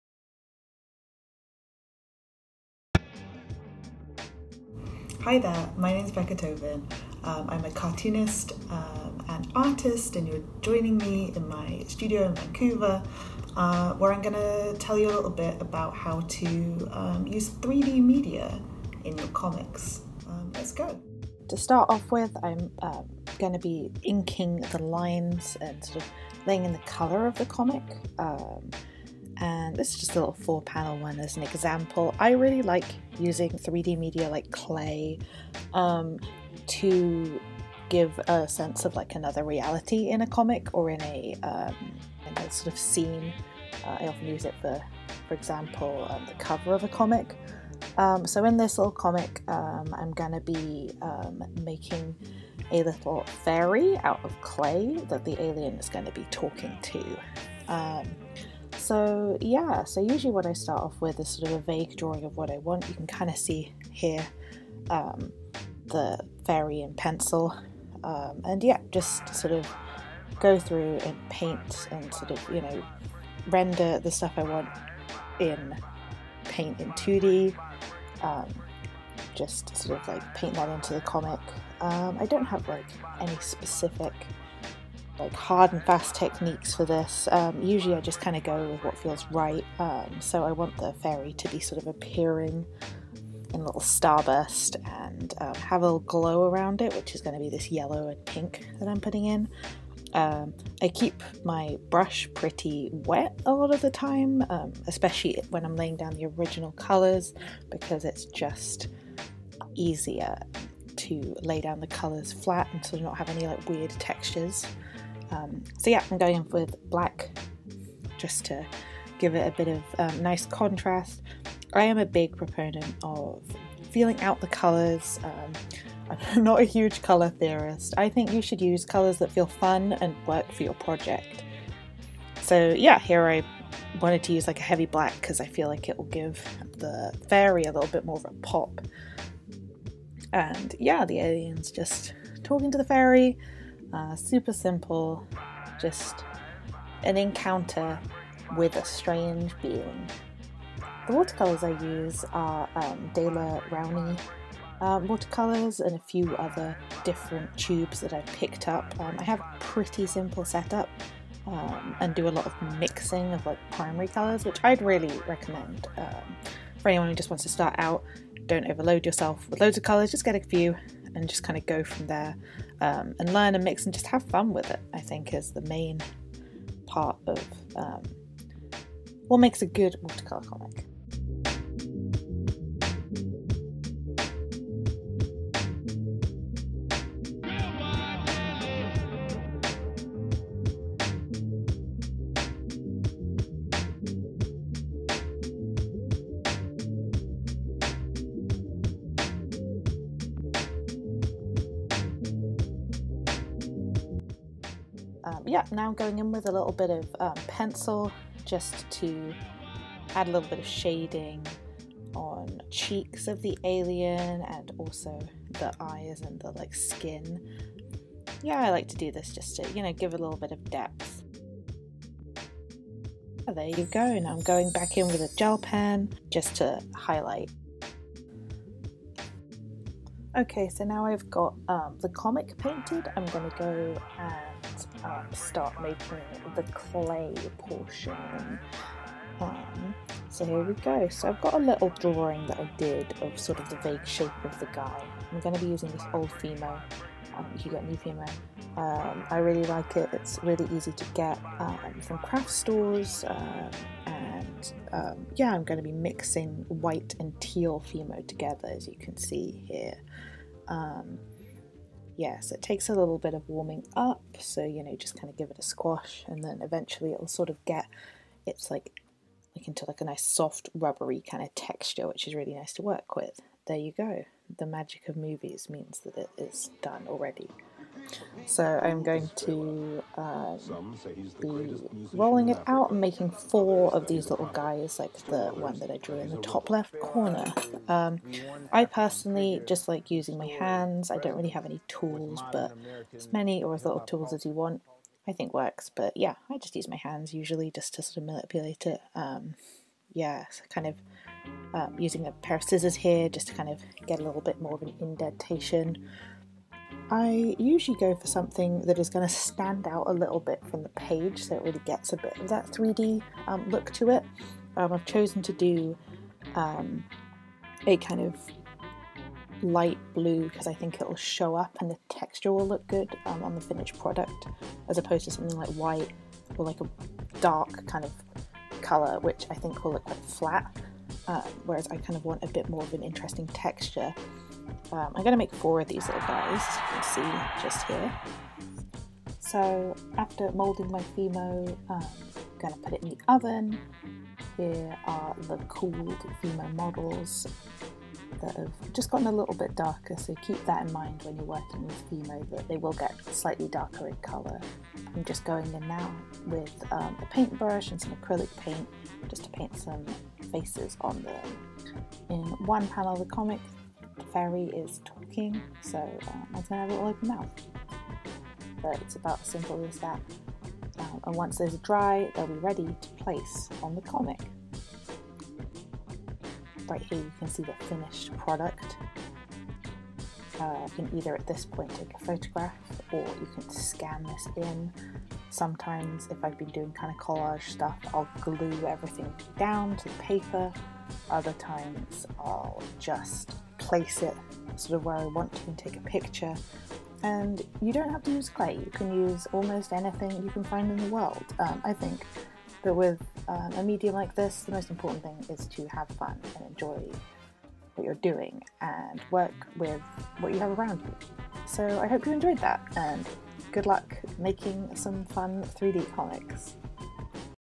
How to Use 3D Media in Your Comics with Becca Tobin
Incorporate 3D elements into your comics!

About Becca:
My name is Becca and I'm a cartoonist, illustrator and visual artist from the UK currently living in Vancouver, BC. My comics include UNDERSTANDING, Frontier 9, It's Me and Lunchtime.

The Vancouver Comic Arts Festival (VanCAF) is a literary festival dedicated to celebrating comics and their creators.